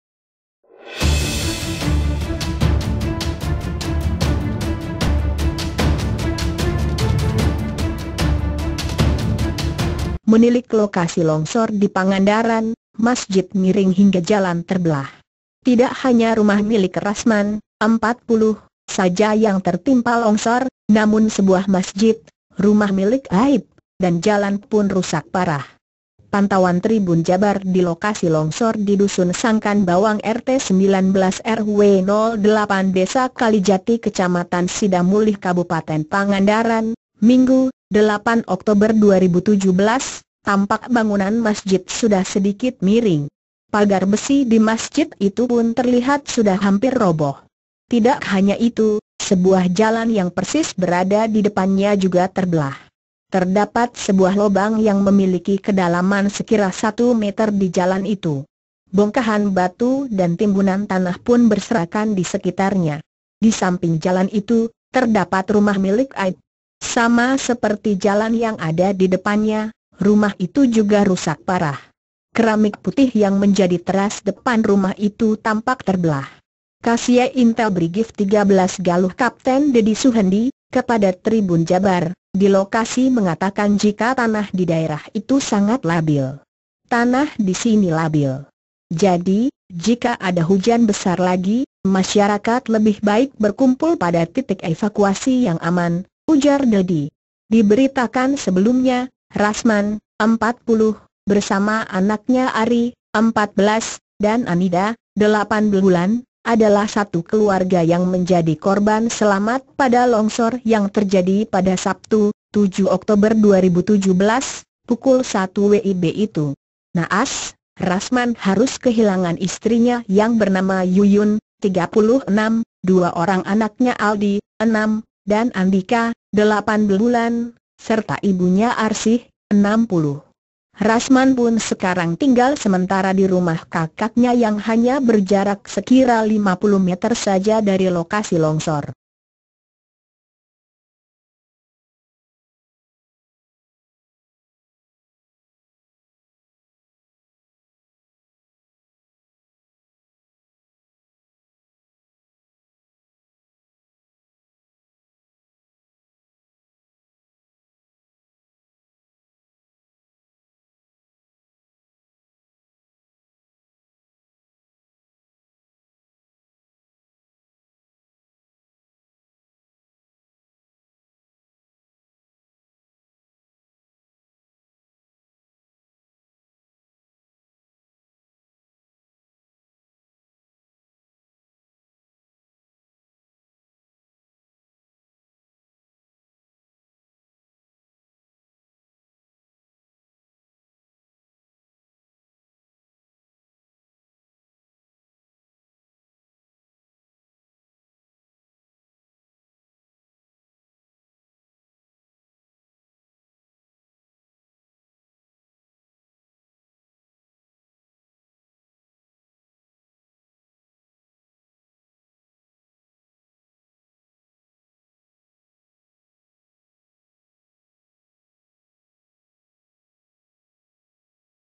Menilik Lokasi Longsor di Pangandaran, Masjid Miring Hingga Jalan Terbelah
Tidak hanya rumah milik Rasman (40) saja yang tertimpa longsor, namun sebuah masjid, rumah milik Aip, dan jalan pun rusak parah.
Pantauan Tribun Jabar di lokasi longsor di Dusun Sangkan Bawang RT 19 RW 08 Desa Kalijati Kecamatan Sidamulih Kabupaten Pangandaran, Minggu (8/10/2017), tampak bangunan masjid sudah sedikit miring.
Pagar besi di masjid itu pun terlihat sudah hampir roboh.
Tidak hanya itu, sebuah jalan yang persis berada di depannya juga terbelah.
Terdapat sebuah lobang yang memiliki kedalaman sekira satu meter di jalan itu.
Bongkahan batu dan timbunan tanah pun berserakan di sekitarnya.
Di samping jalan itu, terdapat rumah milik Aip.
Sama seperti jalan yang ada di depannya, rumah itu juga rusak parah.
Keramik putih yang menjadi teras depan rumah itu tampak terbelah.
Kasie Intel Brigif 13 Galuh Kapten Dedi Suhendi, kepada Tribun Jabar, di lokasi mengatakan jika tanah di daerah itu sangat labil.
"Tanah di sini labil. Jadi, jika ada hujan besar lagi, masyarakat lebih baik berkumpul pada titik evakuasi yang aman," ujar Dedi.

Diberitakan sebelumnya, Rasman (40) bersama anaknya Ari (14) dan Anida (8 bulan) adalah satu keluarga yang menjadi korban selamat pada longsor yang terjadi pada Sabtu (7/10/2017) pukul 01.00 WIB itu.
Naas, Rasman harus kehilangan istrinya yang bernama Yuyun (36), dua orang anaknya Aldi (6) dan Andika (8 bulan), serta ibunya Arsih (60).
Rasman pun sekarang tinggal sementara di rumah kakaknya yang hanya berjarak sekira 50 meter saja dari lokasi longsor.